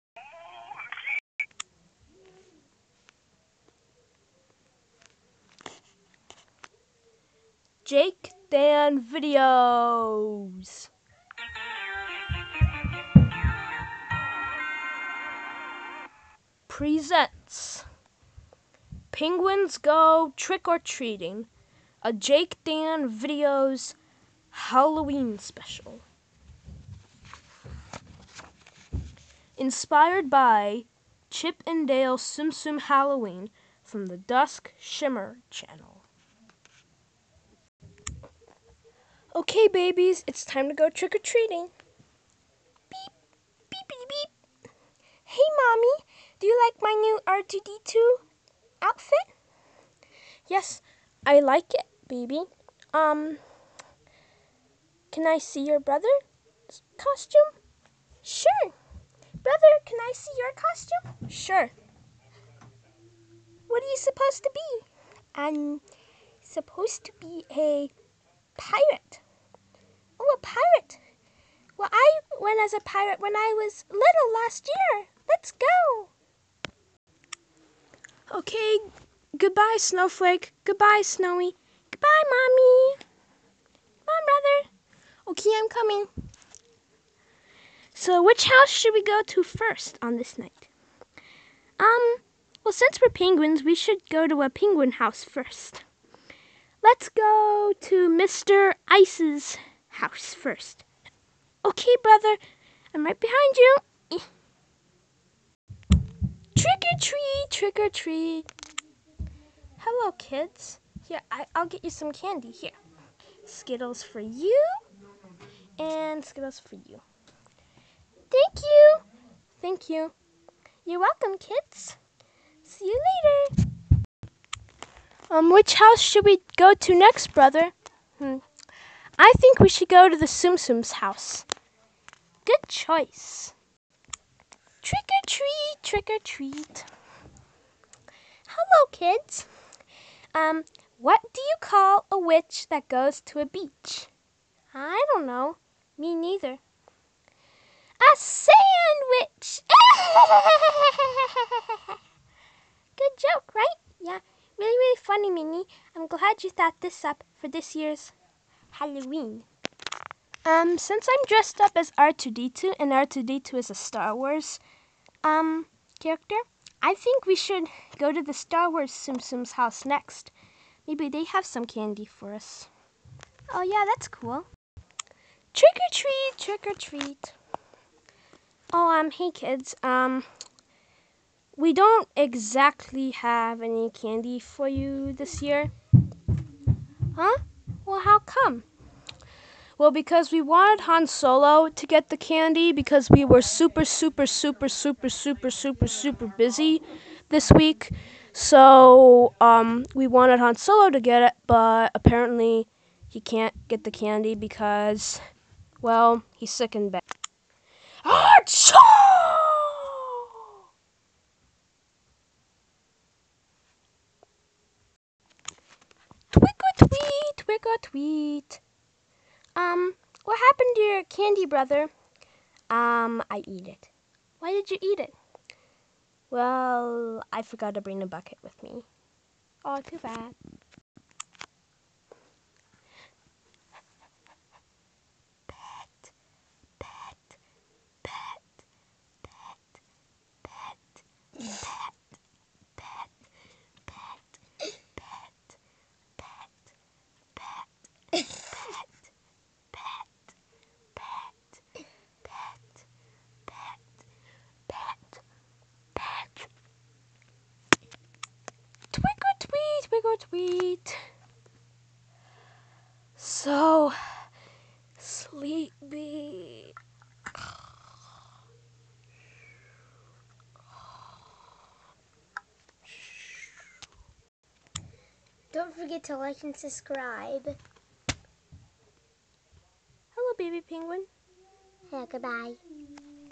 Baby Penguins Go Trick or Treating - Halloween Special
In this video Baby Penguins Go Trick or Treating, well, Baby Penguins Go Trick or Treating. Snowy and Snowflake go out to collect candy from their neighbors and bring you the first halloween special for the JakeDan videos channel.
I hope you enjoy Baby Penguins go Trick or Treating.
Don,t forget to like and wacth more videos on my channel.